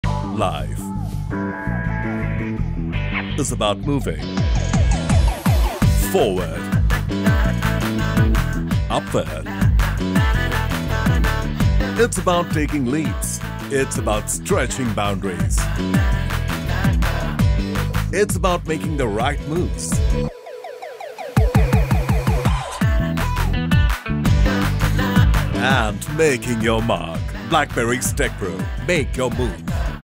MAKE YOUR MOVE | BLACKBERRYS TECH PRO COLLECTION
When it’s about success, every little step counts.

Get the freedom to take that extra step towards comfort and success with our revolutionary TechPro range of trousers, shirts, and more.

Try them now at your nearest Blackberrys store or shop